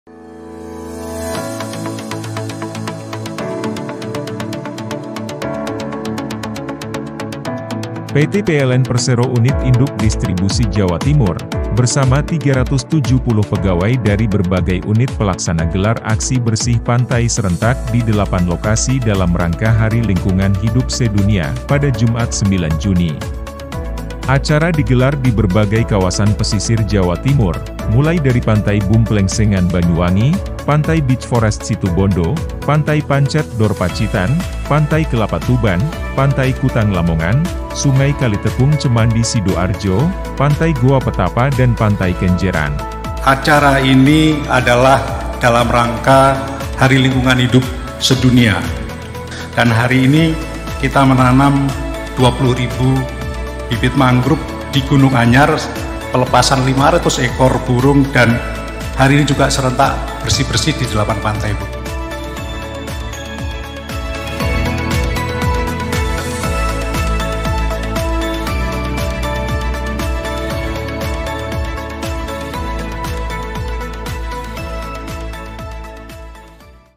Serentak di 8 Lokasi Jawa Timur, PLN Bersihkan Lebih dari 1 Ton Sampah
Sebanyak 1 ton sampah berhasil dikumpulkan pegawai PLN dalam aksi bersih pantai di 8 kawasan pesisir pantai Jawa Timur

Banyuwangi, 10 Juni 2023 - PT PLN (Persero) Unit Induk Distribusi (UID) Jawa Timur bersama 370 pegawai dari berbagai Unit Pelaksana gelar aksi bersih pantai serentak di 8 lokasi dalam rangka hari lingkungan hidup se-dunia pada Jumat (9/6). Acara di gelar di berbagai kawasan pesisir Jawa Timur mulai dari Pantai Boom Plengsengan Banyuwangi, Pantai Beach Forest Situbondo, Pantai Pancet Dor Pacitan, Pantai Kelapa Tuban, Pantai Kutang Lamongan, Sungai Kalitekung Cemandi Sidoarjo, Pantai Goa Petapa dan Pantai Kenjeran.

General Manager PLN UID Jawa Timur mengatakan aksi bersih pantai bertujuan untuk memberikan kontribusi nyata sekaligus edukasi terkait pentingnya menjaga lingkungan bagi masyarakat baik pengunjung maupun UMKM yang ada di sekitar kawasan.

"Melalui kegiatan ini kami ingin membangun kesadaran betapa pentingnya menjaga lestarinya alam termasuk kawasan pesisir. Hal pertama yang kami lakukan ialah dengan menggerakkan pegawai dan menggandeng stakeholder serta masyarakat untuk terlibat dan ambil peran," tutur Lasiran.

Ia menambahkan luasan total 8 kawasan yang dibersihkan 1,2 hektar dan mampu mengumpulkan kurang lebih 1 ton sampah dari berbagai kawasan tersebut.

"Jika ditotal penerima manfaat sejumlah 1.159 berasal dari masyarakat dan UMKM sekitar kawasan. Kami juga bersinergi dengan Dinas Lingkungan Hidup, lembaga swadaya masyarakat, komunitas, pokdarwis dan lainnya," imbuh Lasiran.

Di Banyuwangi, lokasi aksi bersih pantai dilaksanakan di Pantai Boom Plengsengan yang merupakan salah satu destinasi wisata di Banyuwangi, tetapi belakangan keindahan pantai ini dipenuhi sampah plastik. Diawali dengan konvoi motor listrik, pegawai Unit Pelaksana Pelayanan Pelanggan (UP3) Banyuwangi menuju lokasi dan melakukan aksi bersih pantai.

“Terimakasih atas kegiatan bersih-bersih pantai yang telah dilakukan oleh PLN Banyuwangi, diharapkan dengan adanya penyerahan tempat sampah dan lingkungan pantai yang sudah bersih ini dapat meningkatkan kepedulian masyarakat untuk menjaga kebersihannya,” ucap Ketua Bagian Keamanan Pantai Boom Plengsengan Banyuwangi, Ramli.